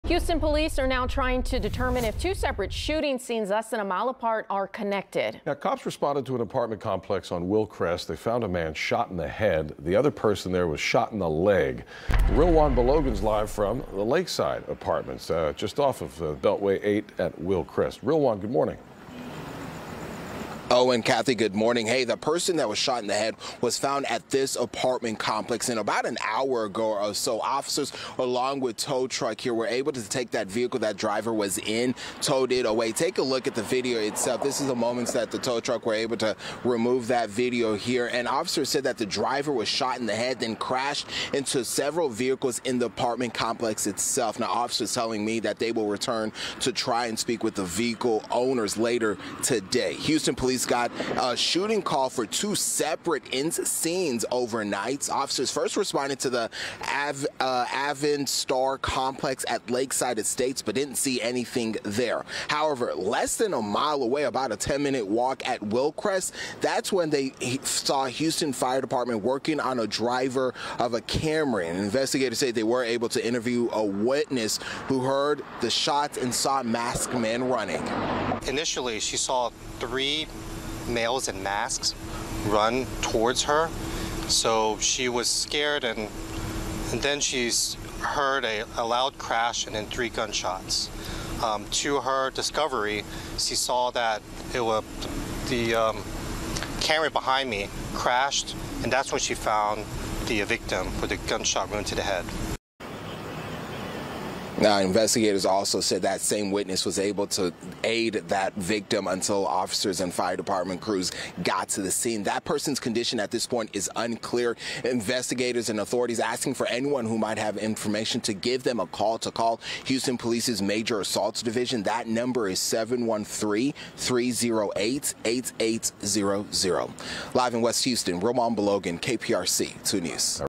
HPD investigating 2 shootings in west Houston
Authorities with the Houston Police Department are trying to determine if two separate shooting scenes are connected.